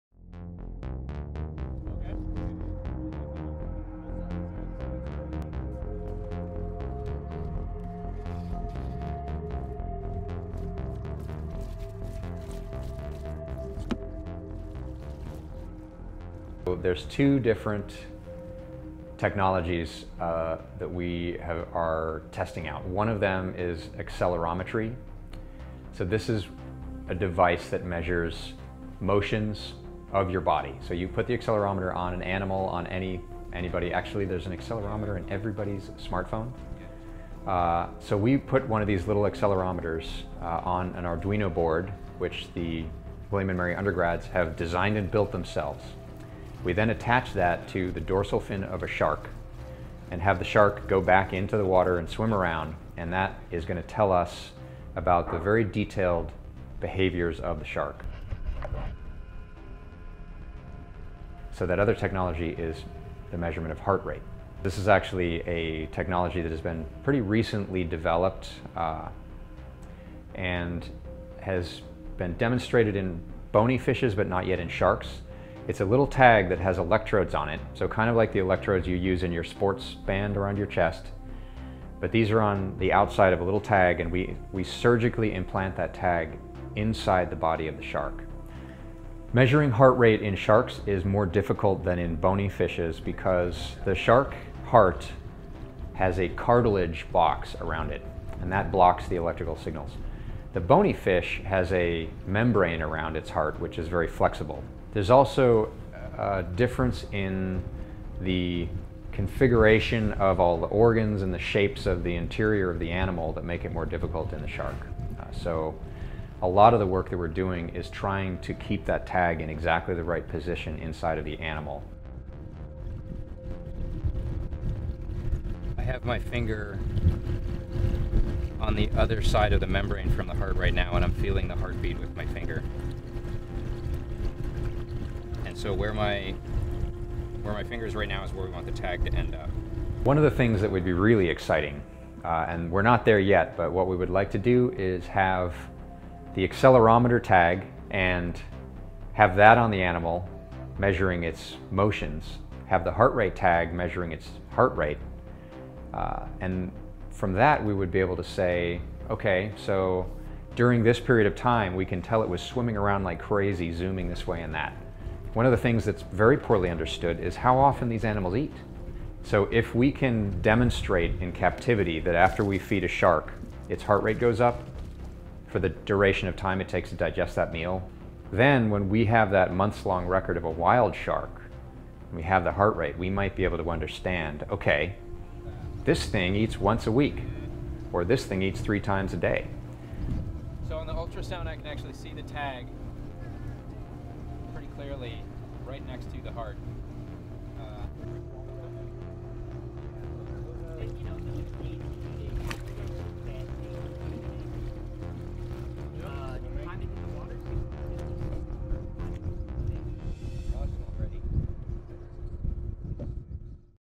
Shark research at Wachapreague
Professor Kevin Weng describes his ongoing research with sharks at the Eastern Shore Lab of the Virginia Institute of Marine Science (VIMS).